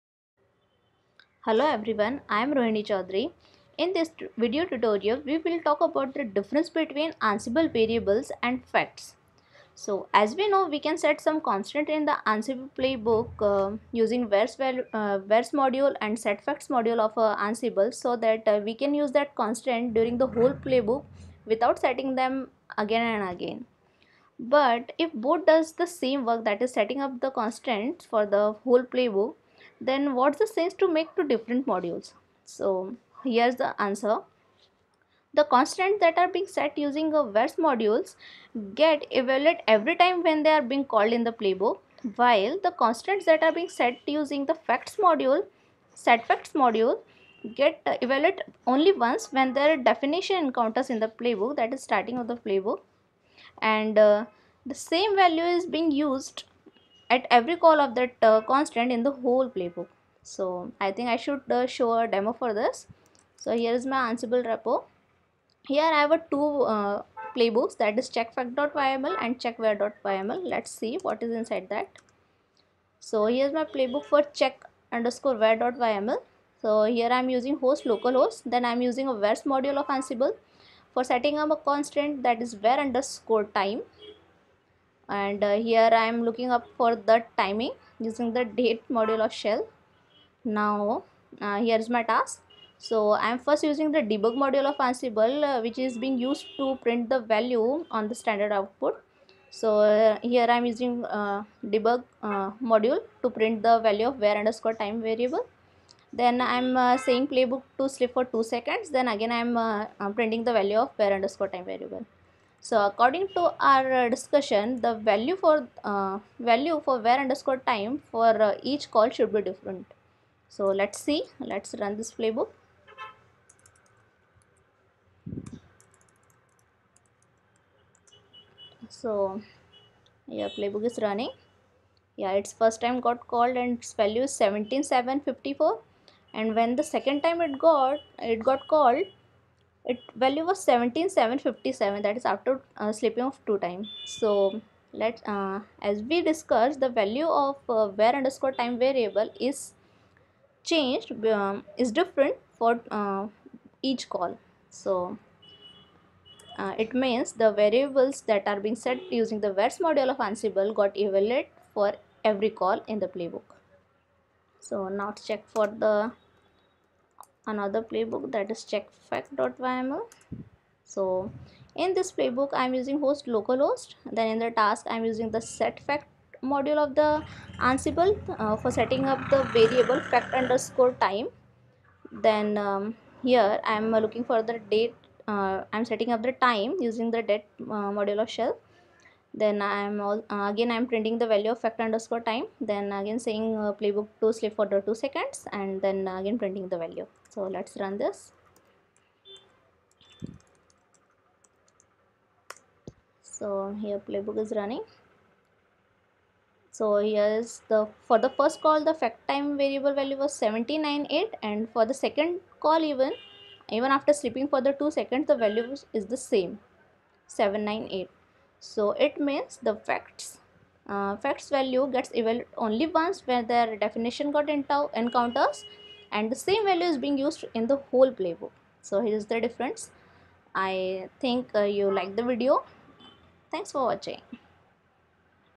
Difference between Ansible vars and set_fact module | Ansible Tutorial for set_fact module and var
This Video tutorial explains the actual difference between the vars and set_fact module of Ansible and explains how they work under the hood.